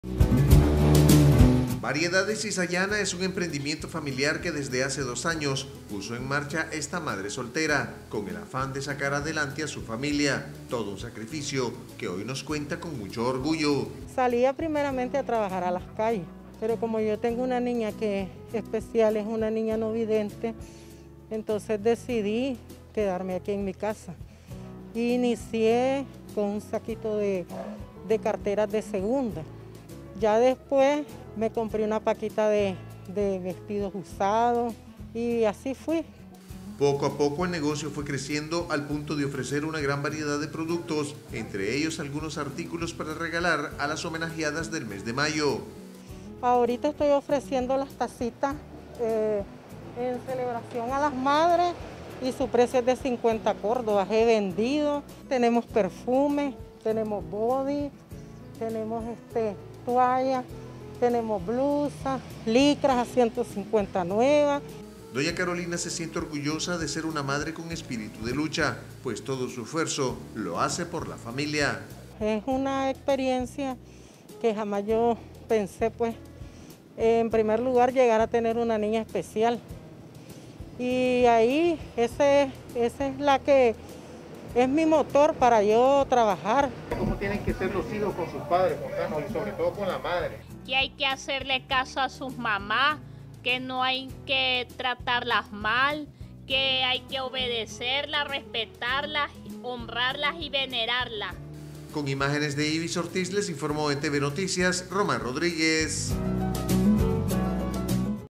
MUJER Y MADRE CON ESPÍRITU DE LUCHA
Carolina del Socorro Castro Leiva, es una madre luchadora de la ciudad capital, con mucho empeño y dedicación ha logrado establecer un pequeño negocio donde ofrece una variedad de productos...